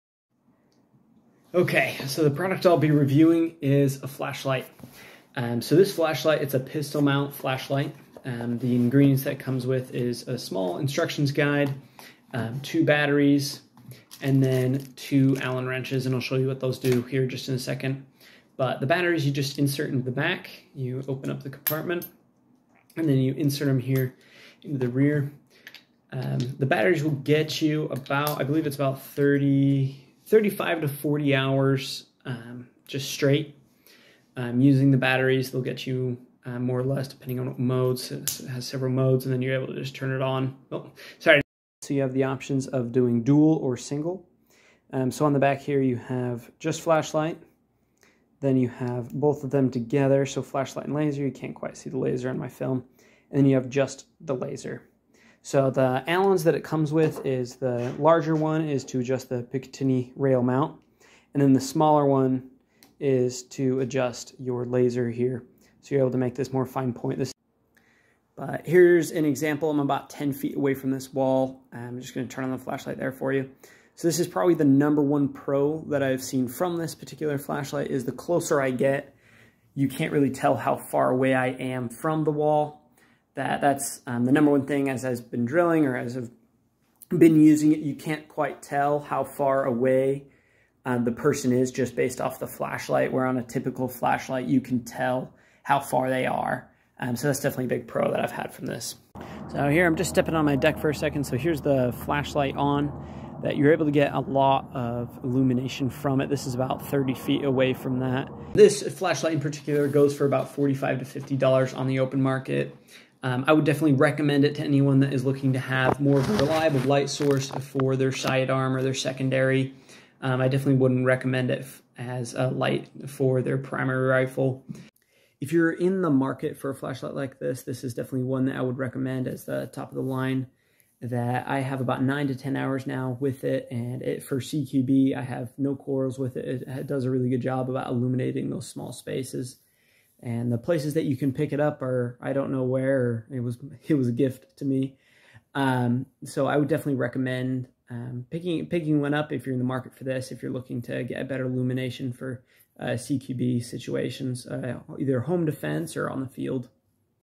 Feyachi Pistol Light and Laser Combo
Isaac "Rounder" Howells reviews a Pistol Light/Laser combo by Feyachi that hits the mark for him.  It has a strong beam, easy controls, and gives a ton of light when you need it.